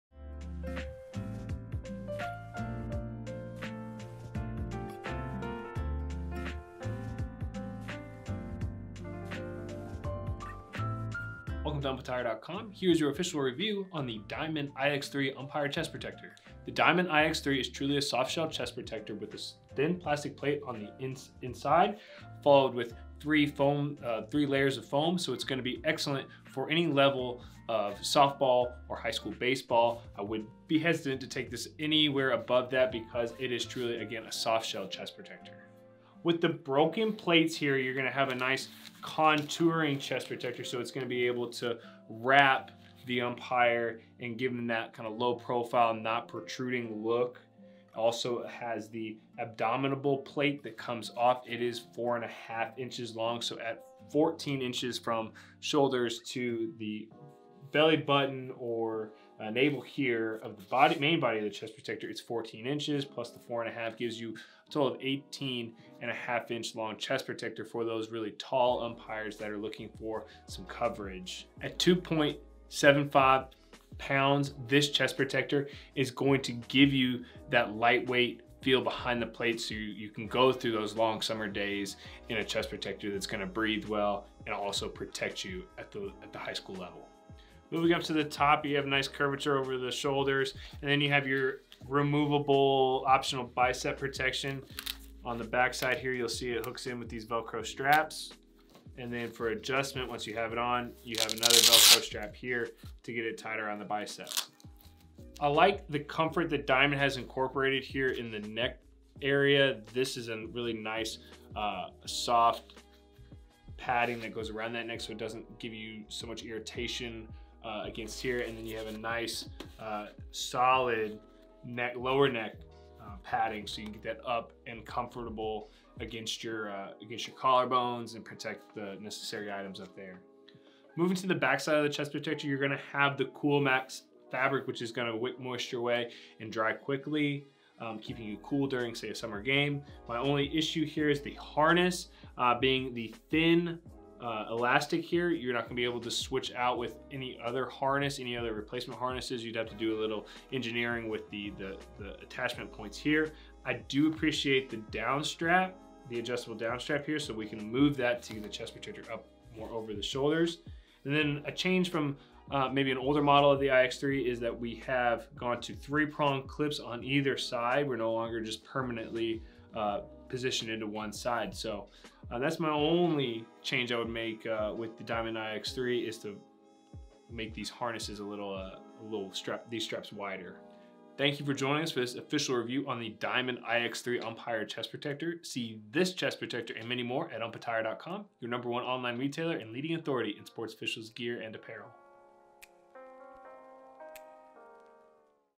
Official Review: Diamond iX3 Umpire Chest Protector
Lightweight, moldable, breathable, and protective. See why we recommend the Diamond iX3 Umpire Chest Protector.